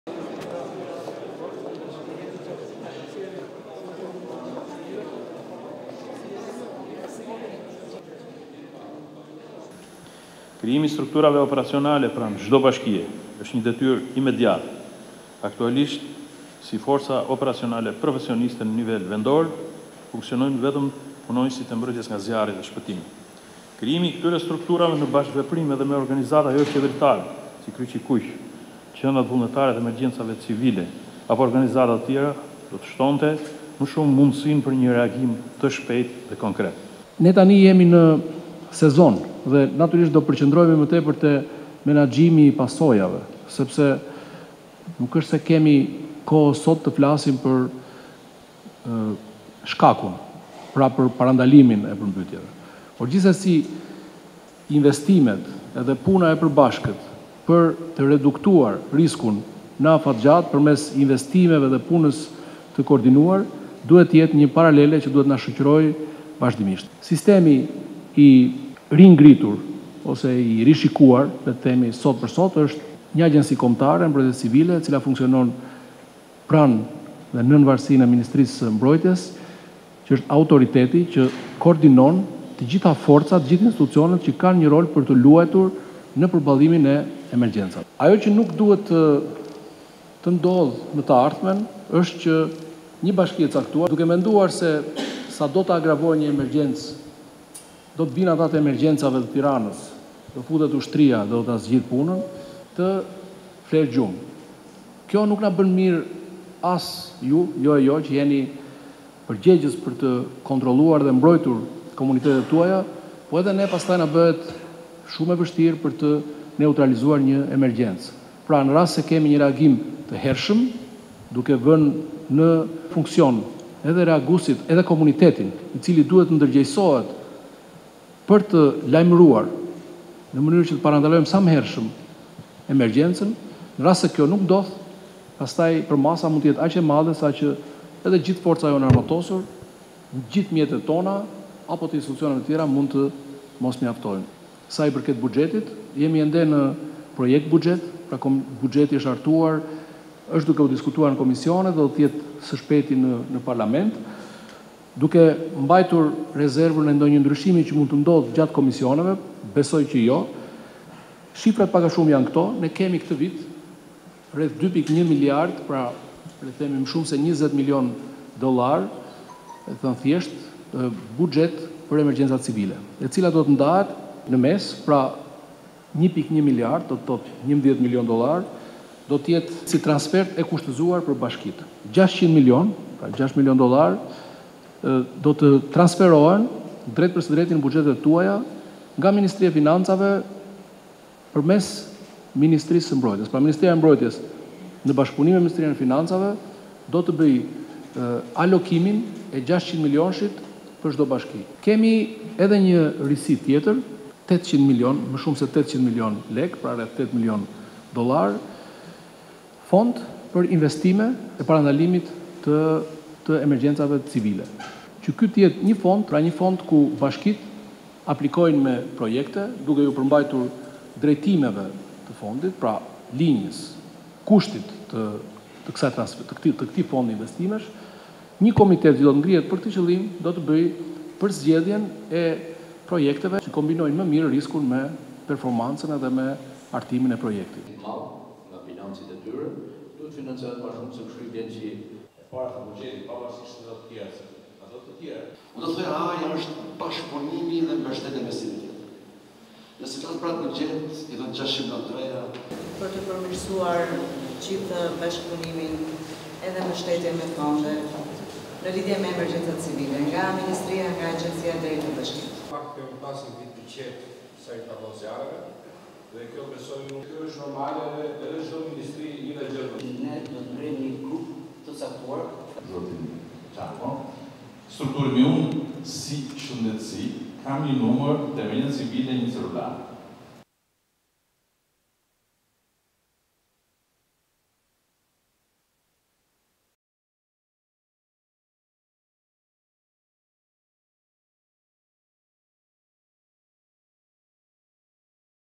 Qeveria mbi 20 mln dollarë në buxhetin e ri për Emergjencat Civile, Ministri i Mbrojtjes: Koordinim
Bashkitë e vendit do të kenë mbështetje mbi 20 milionë dollarë në buxhetin e ri për Emergjencat Civile, para që do të përdorues specifikisht vetëm për emergjenca. Për përballimin e përmbytjeve gjatë sezonit të dimrit, ministri kërkoi një bashkëpunim dhe koordinim më të mirë mes bashkive dhe institucioneve qendrore.